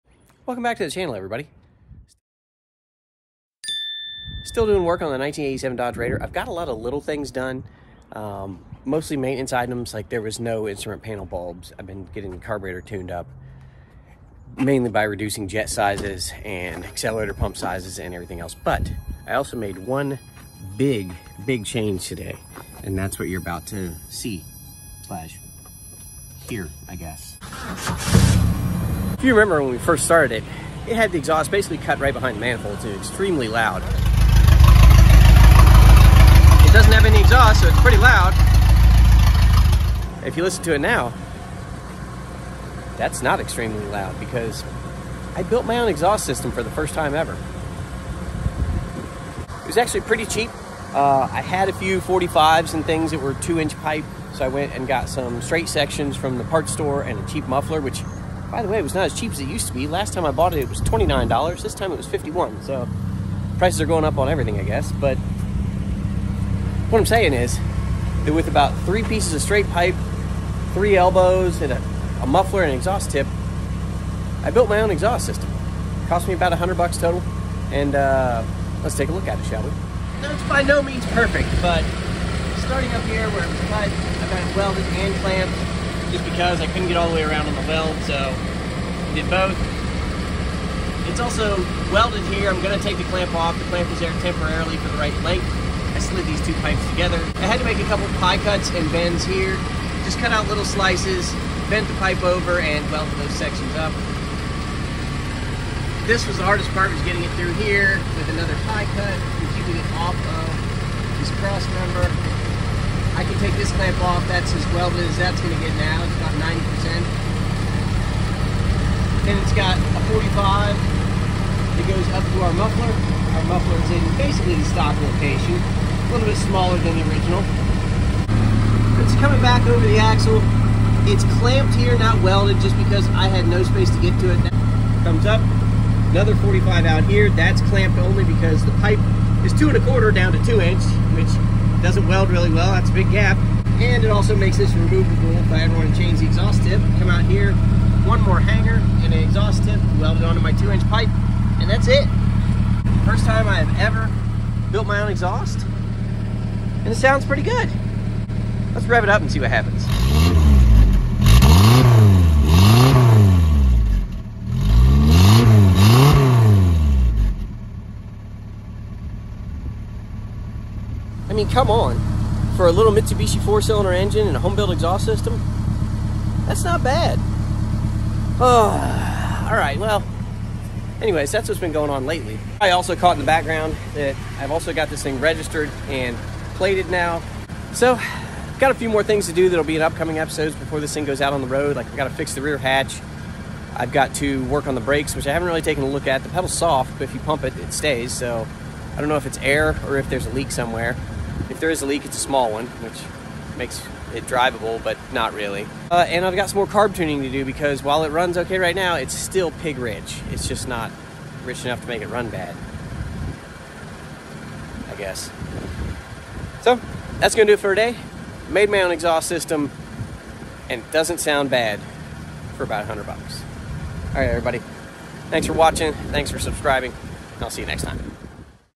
I made a Full exhaust system for $100!
They no longer make the exhaust for my 87 dodge raider (1st gen Mitsubishi Montero or Pajero).  Mine has bee lopped off at the end of the down pipe so I have to make my own….for cheap.  Well, here goes nuthin’.